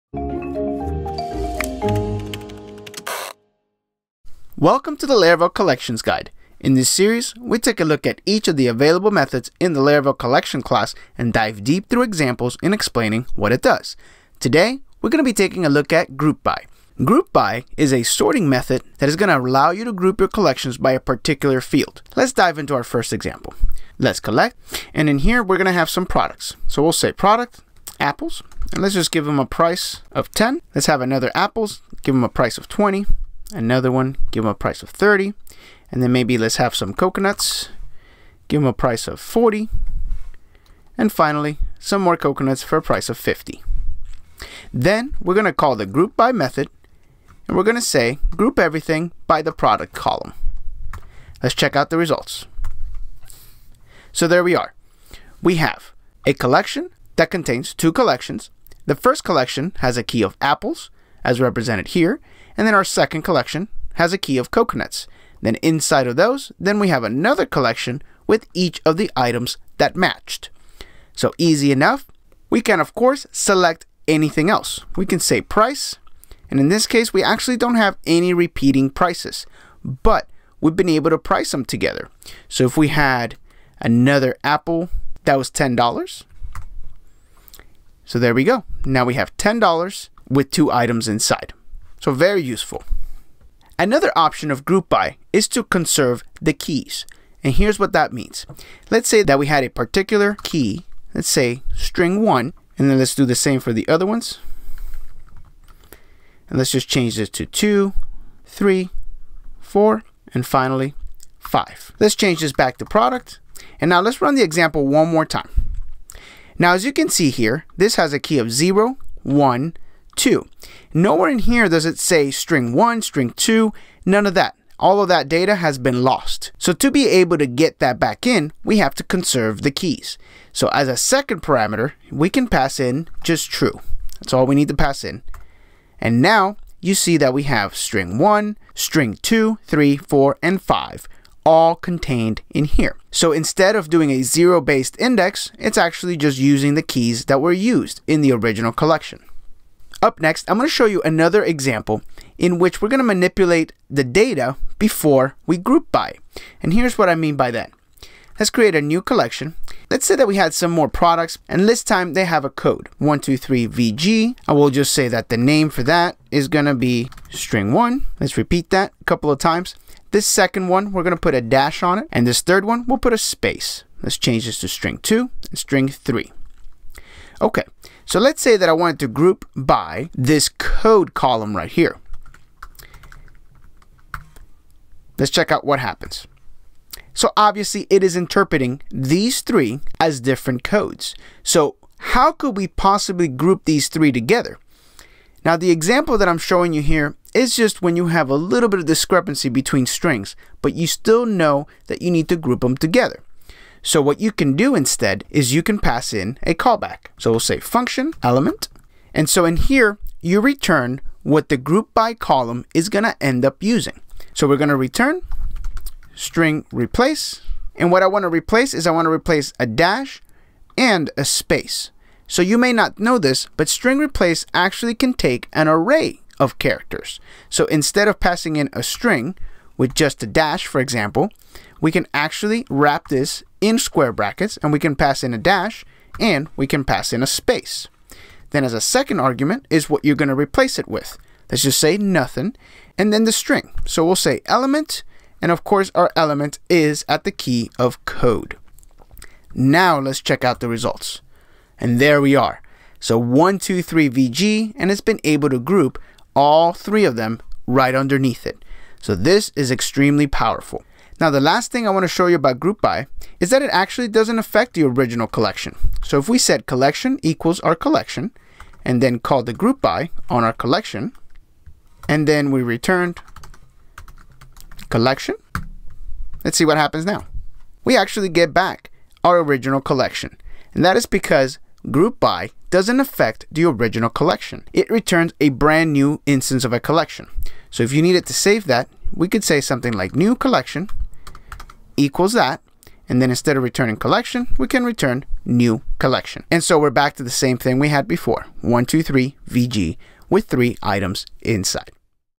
groupBy | Laravel Collections
When the need arises to pivot your collections by a field, the groupBy() method comes to the rescue. For more advanced grouping, check out the callback features.

In this series, we take a look at each of the available methods in the Laravel Collection class and dive deep through examples in explaining what it does.